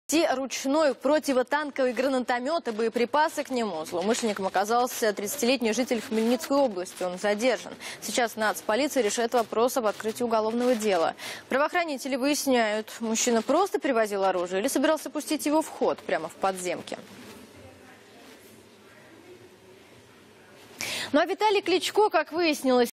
В Киеве задержан человек с гранатометом
У жителя Хмельницкой области в Киеве обнаружили переносной ручной гранатомет